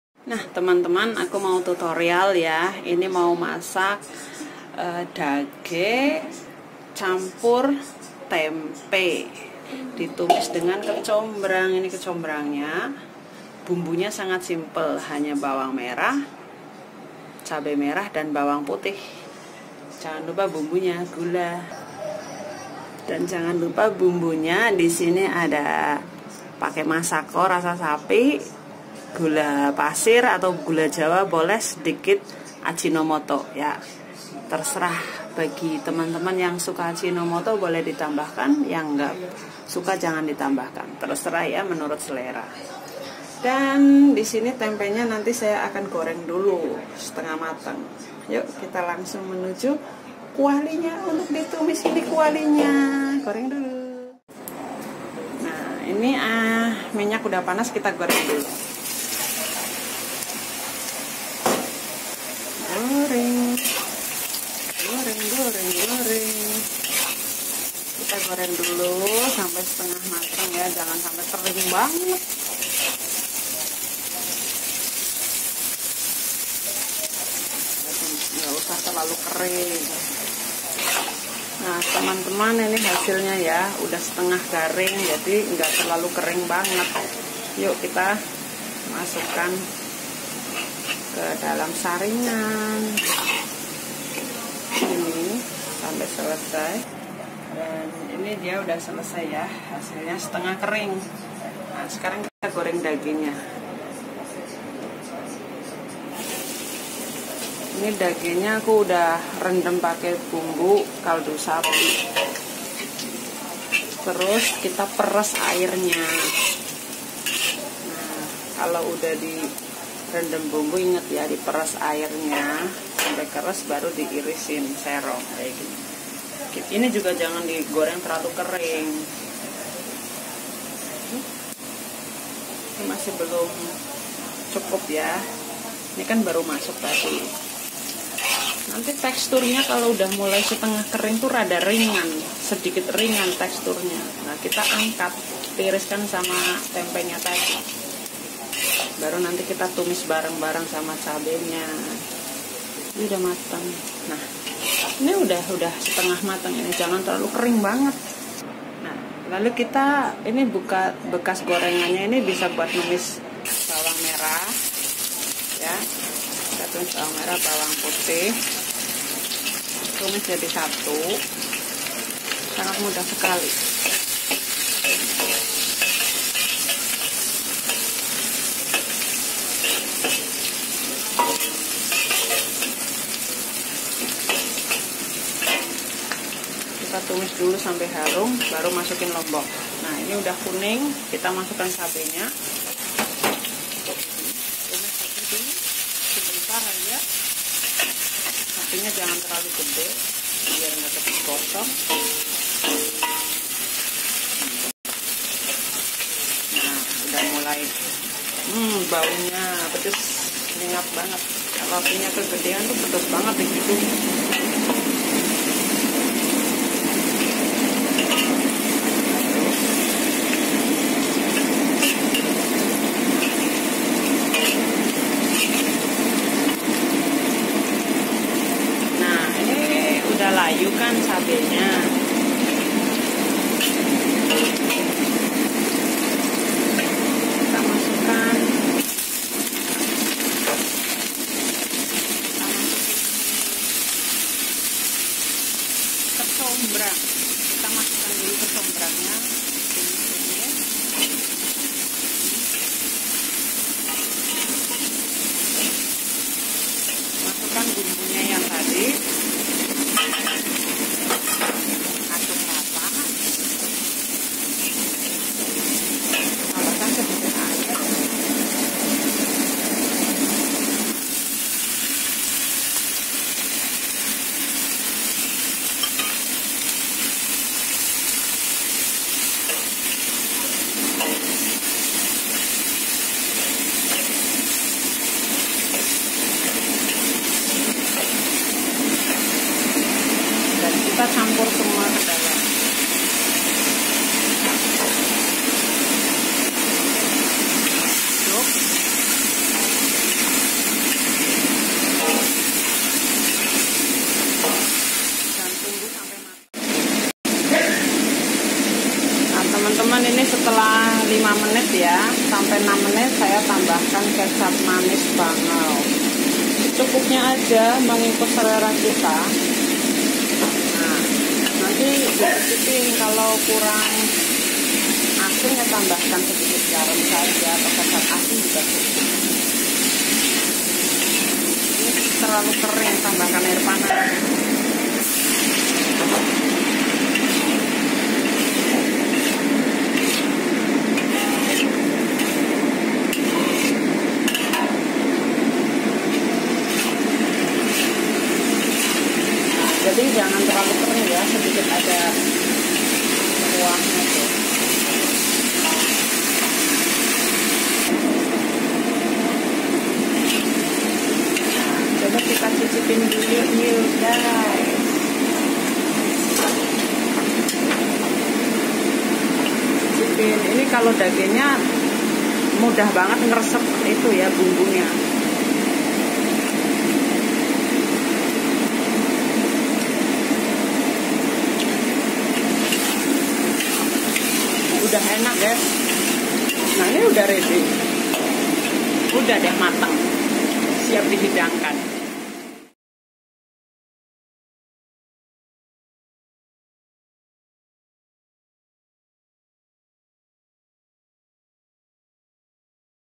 Tumis Dages Dan Combrang
Bahan
Combrang
Dage
Tempe
Cabe
B merah
B putih
Gula
Garam
Ajinomoto
Dan kecap manis cap bangao